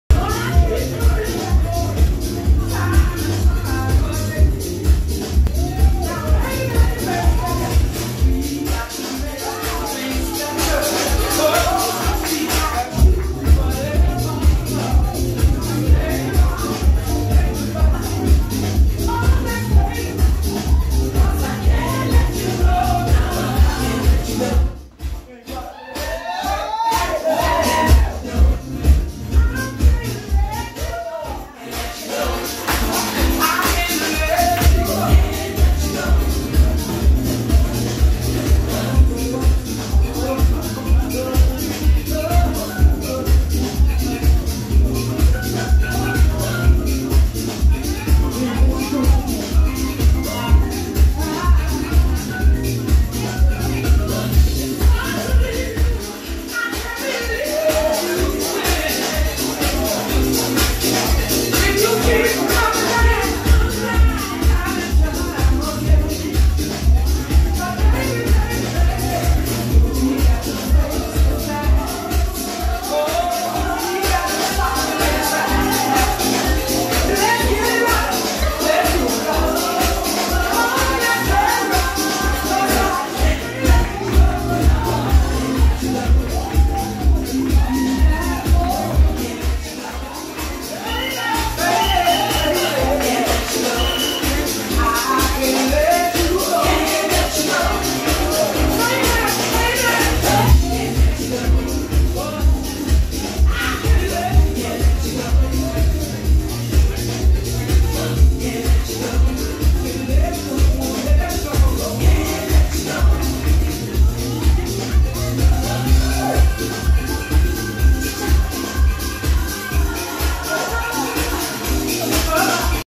Frankie Paradise at the Soul Committee Alliance Memoral Day Weekend Event
Frankie Paradise have the crowd in a trance at the Soul Committee Alliance Memoral Day Weekend Event